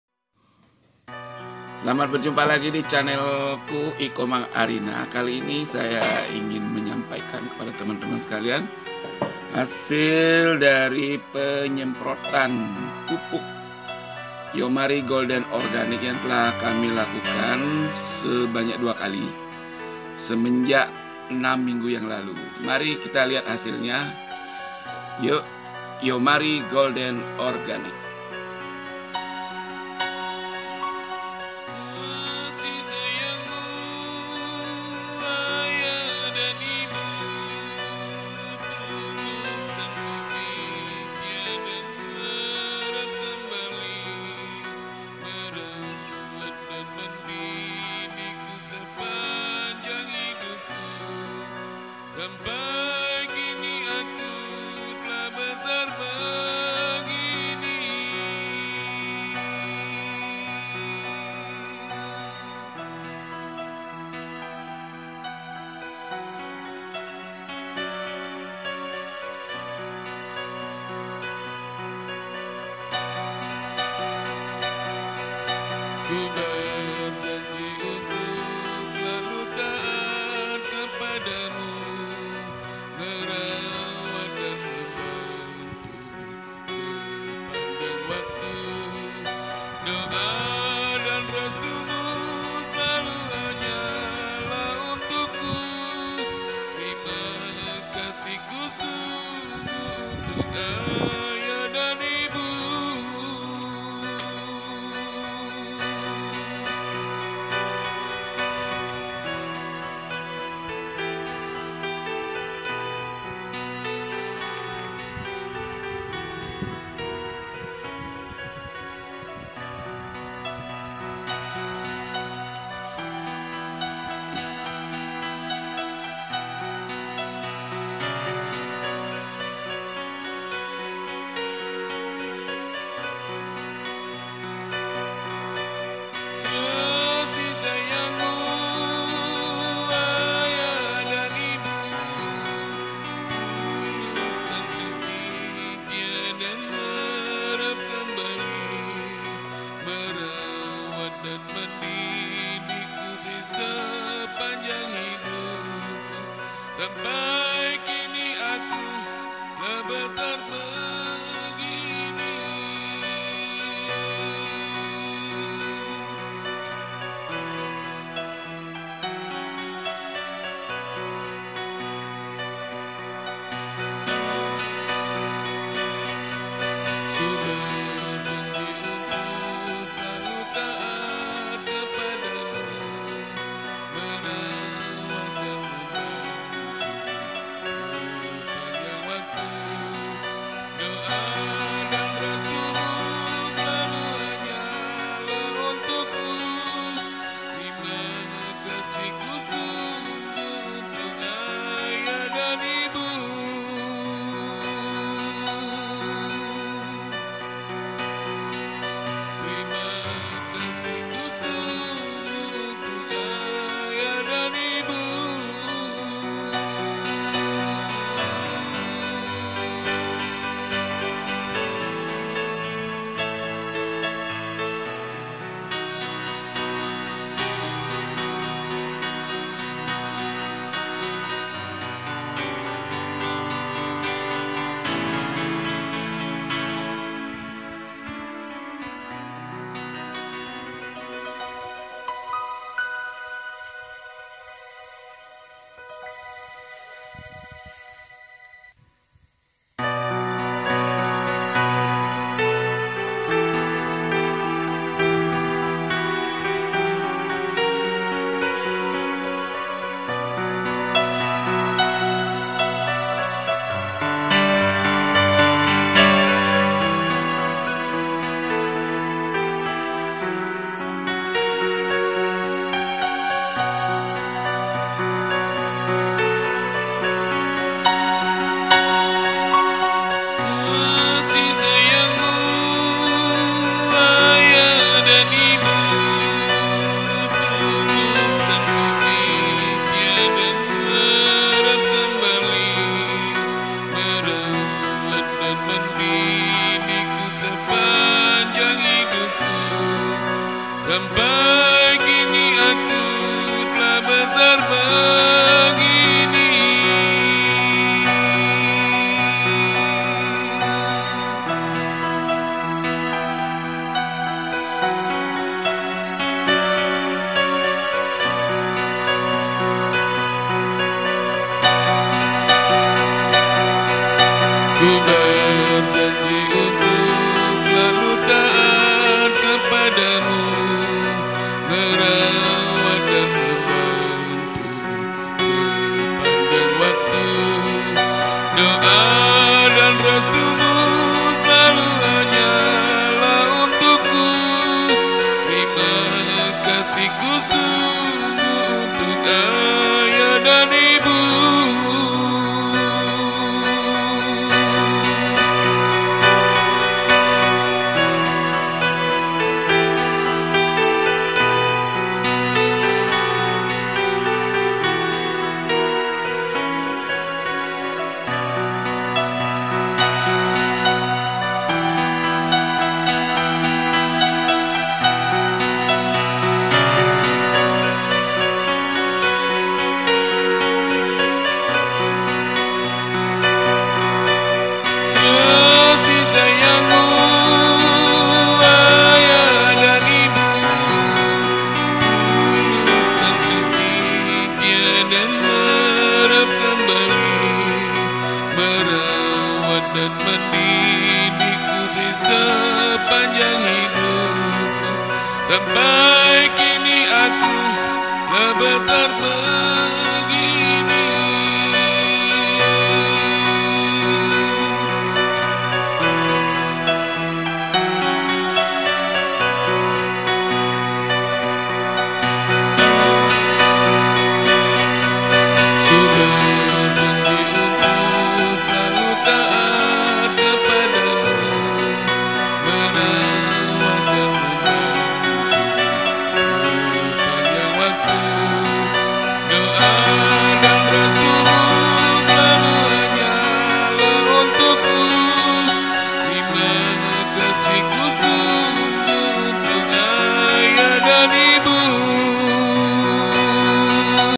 Yomari Golden Organik pada durian muda . 6 minggu setelah semprot
tanaman apapun sangat perlu nutrisi seimbang sesuai kebutuhan.  YOMARI GOLDEN ORGANIK  jawabannya.  terbukti di kebun saya,  sudah 1 kali semprot.sejak 6 minggu terakhirnya bermakna  pertumbuhan nya